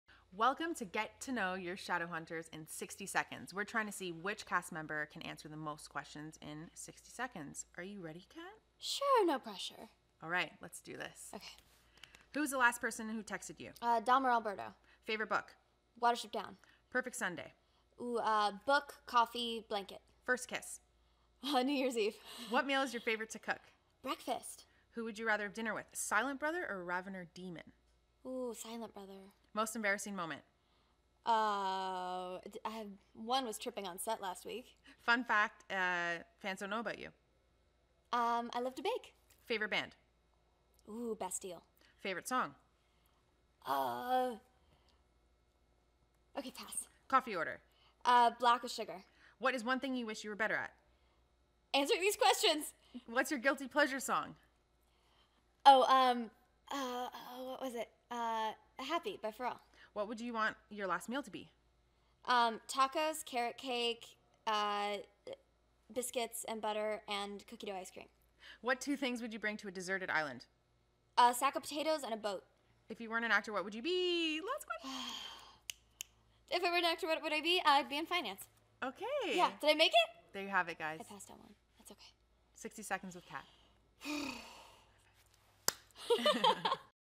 Shadowhunters 60 secs game w Kat Mcnamara
Do you think you know Kat? Here are some thing you didn't know about him in the 60 seconds game! Gret work Kitkat...only one pass...=D

Don't miss Shadowhunters's Summer premiere, Monday JUNE 5th 8/7c on Freeform and worldwide Tuesday midnight PST on Netflix